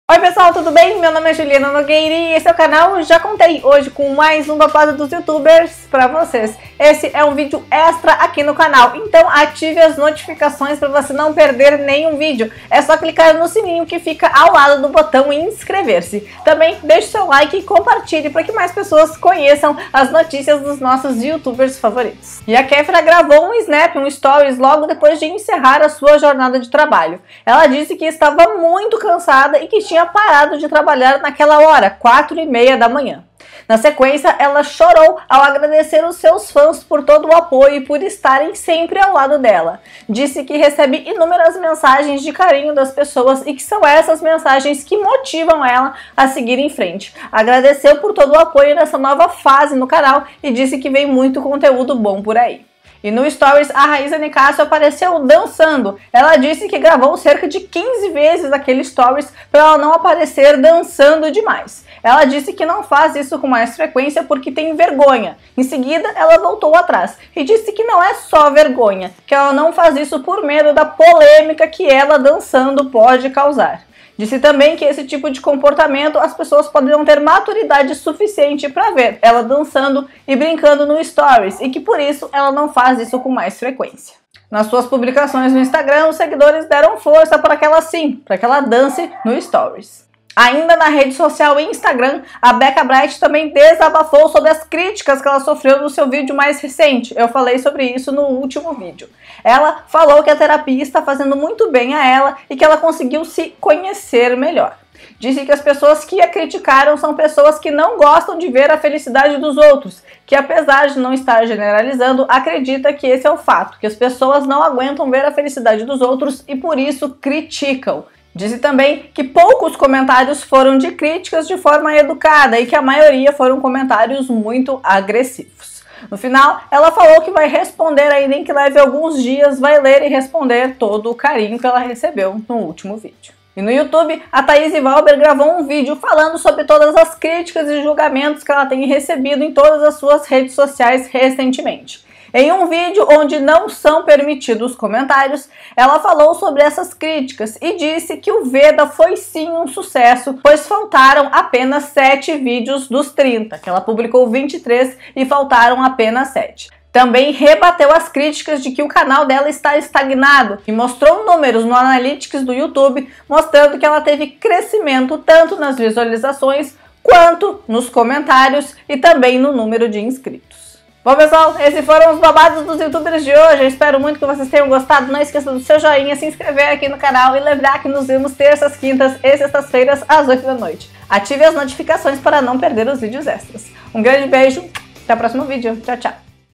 "Não danço para não dar polêmica" diz Rayza Nicácio, Beca e Taise rebatem CRÍTICAS e Kefera chora!
📍Estou aqui Terças, Quintas e Sextas às 20h,  ATIVE as notificações, assim todo vídeo EXTRA você não perde!
Confere a minutagem👇🏻
0:22 Exausta Kefera chora no Instagram
0:53 Rayza Nicácio diz não dançar para não causar polêmica
1:31 Beca fala sobre repercussão do vídeo sobre nova fase do canal
2:13 Taise Walber fala sobre críticas e julgamentos recebidos recentemente e diz que VEDA foi sucesso
❌Dá uma olhada, NÃO PERDEU NENHUM BABADO? 🤔

SNAPCHAT sevenmakeup